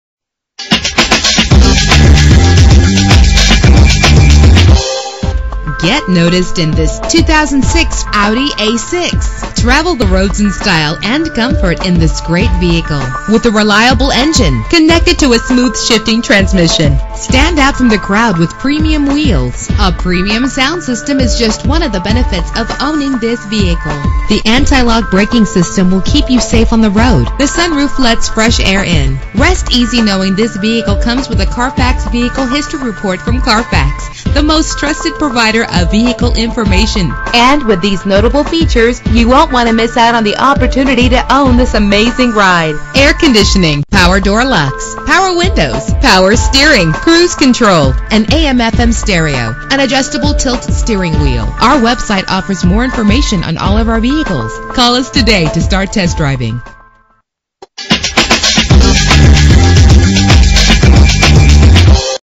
2006 Audi A6 - Whitefish MT
Year: 2006
Make: Audi
Model: A6
Trim: 3.2 Quattro Sedan 4D
Engine: 6 cylinder 3.2 Liter
Transmission:
Color: Gray
Mileage: 76266
Address:
6331 HWY 93 South